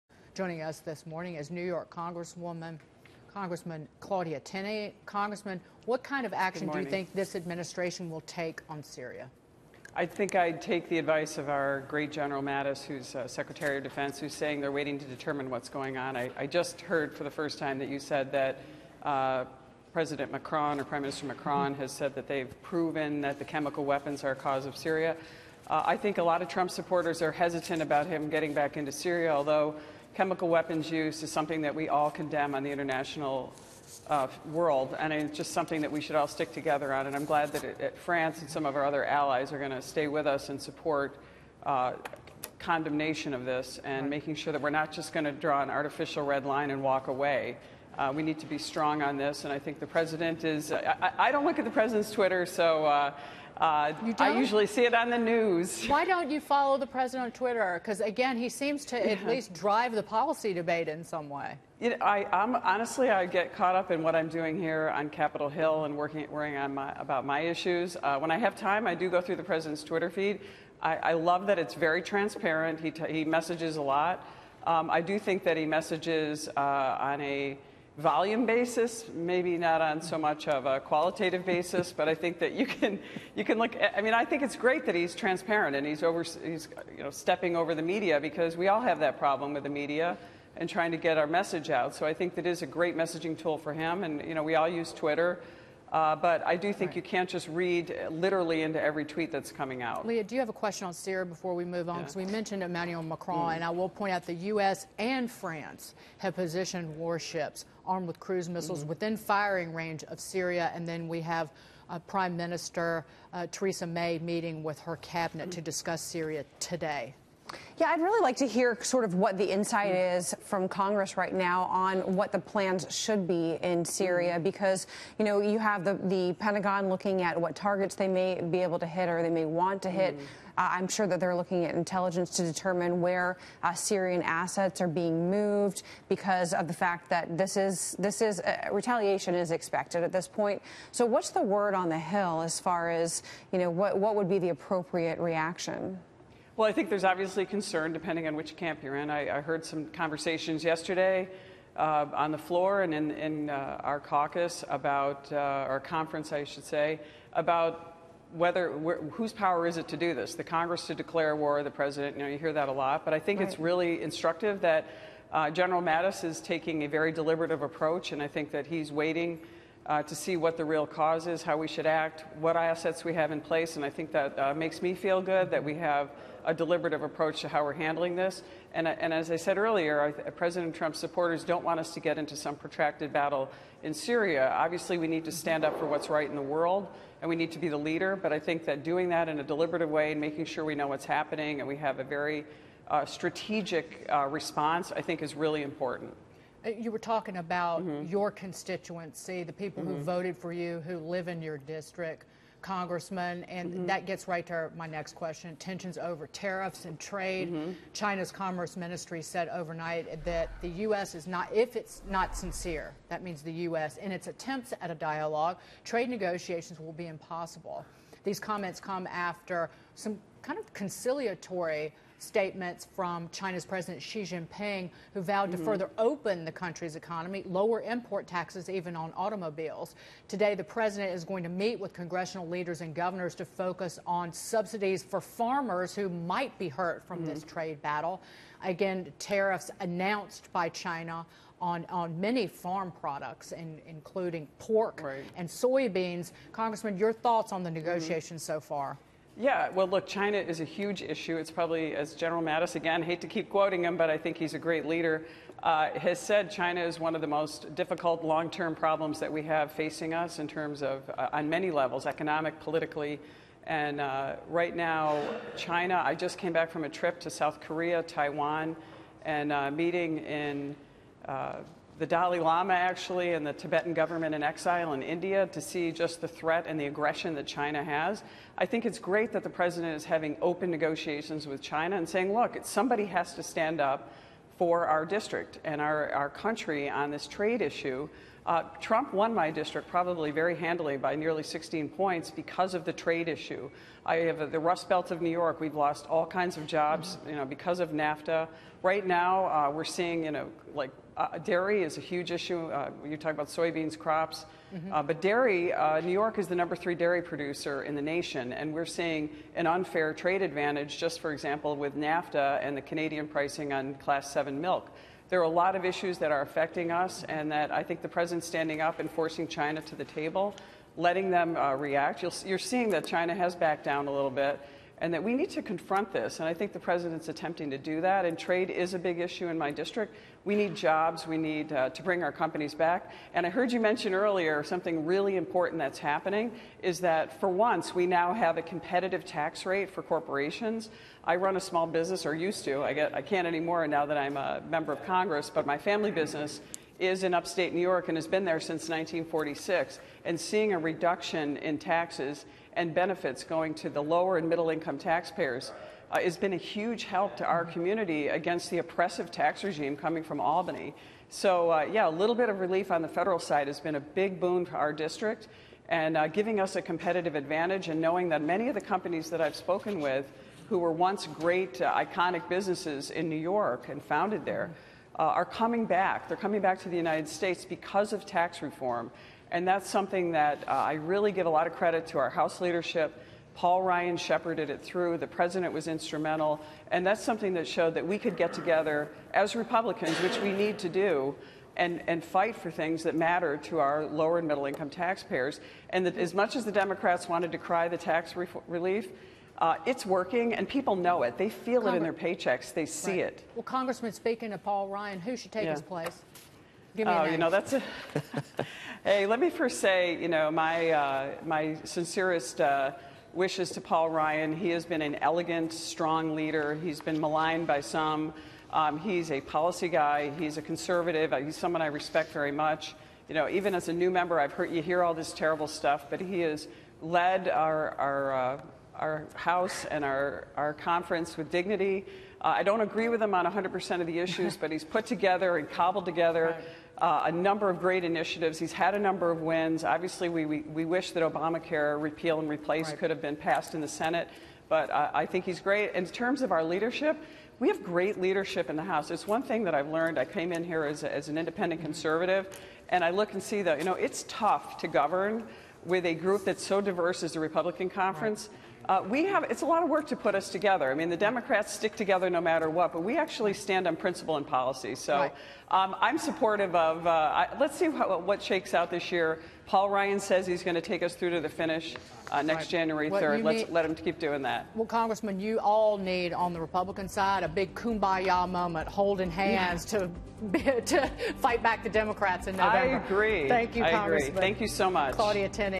It’s great Trump is having open negotiations with China: Rep. Tenney
Rep. Claudia Tenney, (R-N.Y.), on Syria, the Trump administration's trade policies and the tax reform legislation.